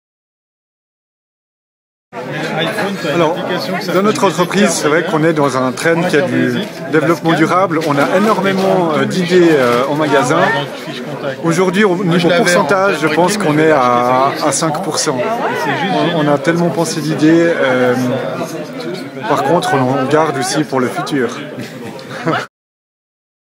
CSID-IDEADETECTIVE-IDEASDONE-BALEGNO-5-2010-FR
Vincent Balegno, head of sales and marketing of Geroco  in Martigny, answered to the question in French: "What percentage of ideas are actually carried out in your company?".